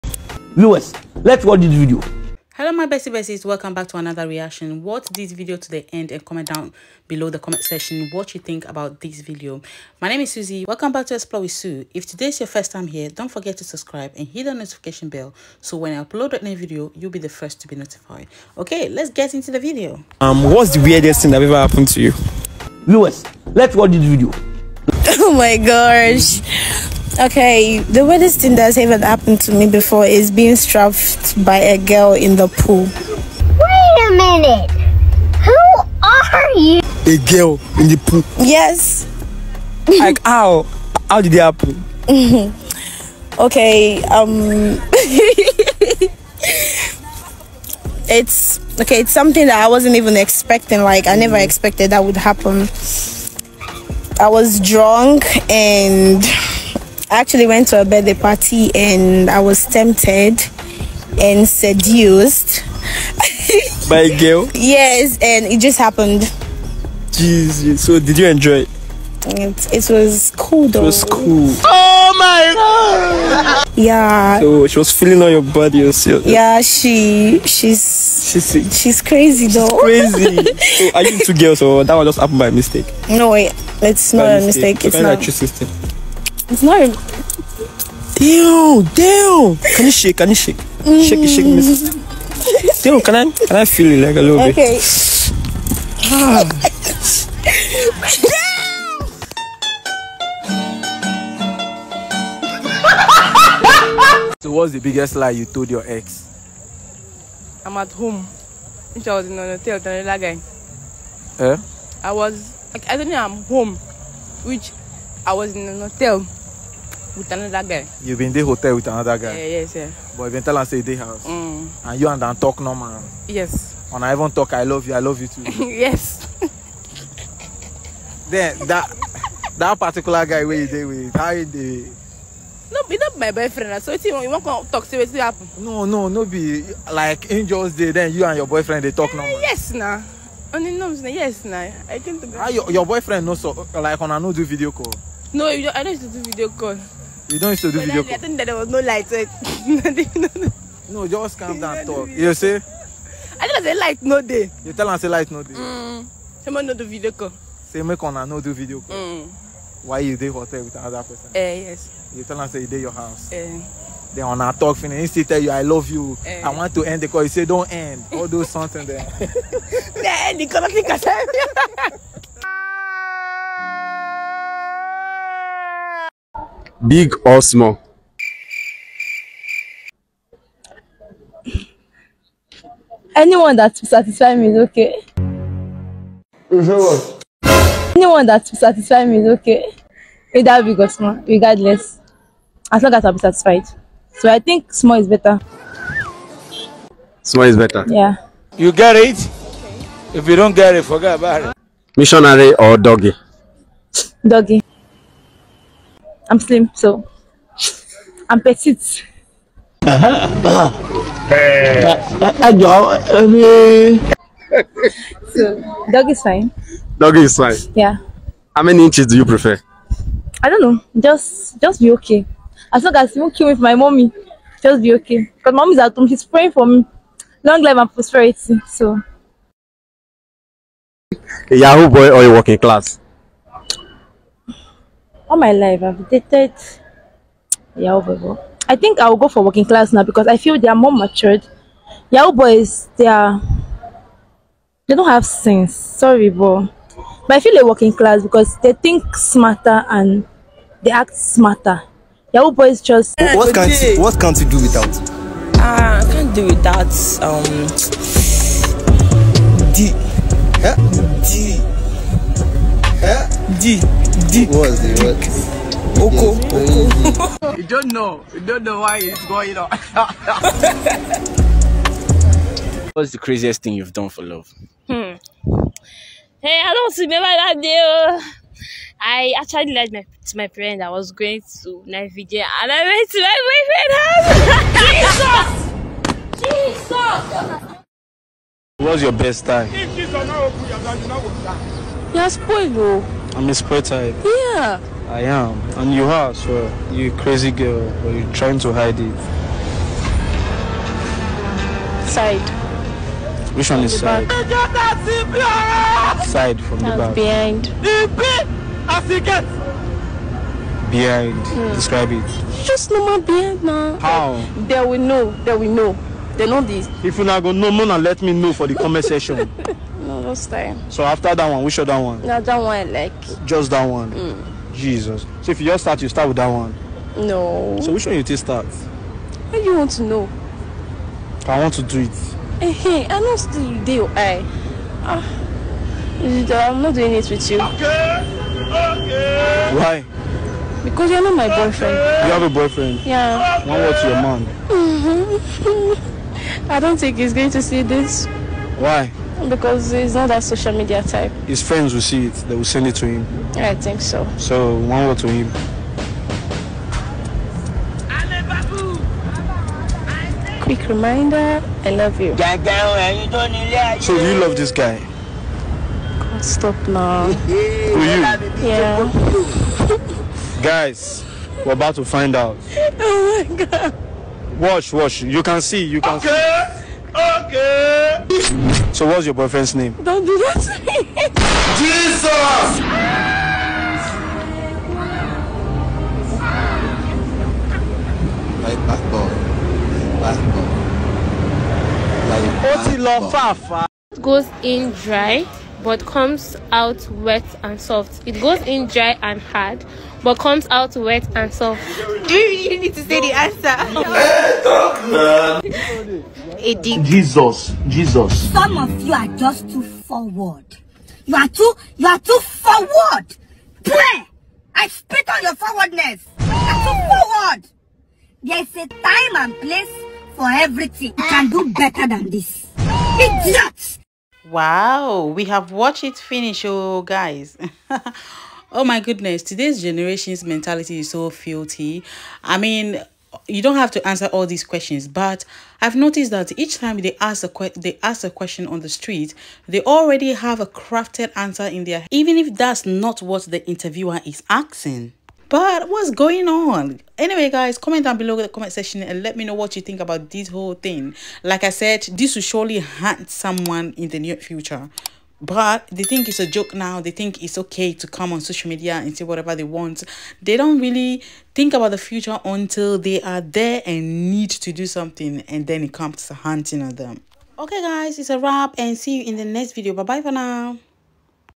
UNBELIEVABLE: These Girls Have Lost It - A Must-Watch Video!
✨🌟 WHAT…HOW… WHO…WHY!

Youngsters Uploading inappropriate content, Sharing personal information with the public can deter their chances in the future

✨🌟 VIDEO Chapters

Wha girls of today are up to   - 00:00
Intro  - 00:10
Explorewithsu   - 00:20
Wirdest things  - 00:37
Is it ok to keep male/female beaties while in a relationship?  - 01:37
Guessing u*dies for a K!ss  - 02:25
Fat or flat - 03:51
Craz!est thing under the rain - 04:50
Red flag in a man? - 05:53
Cra!est thing you never told your Ex - 07:14
+18, names dey - 08:28
Conclusion and Call to Action -10:10

✨🌟KEYWORDS/ HASHTAG

✨🌟MUSIC
🎶 Copyright-free Music from - Pixabay (Intro Music)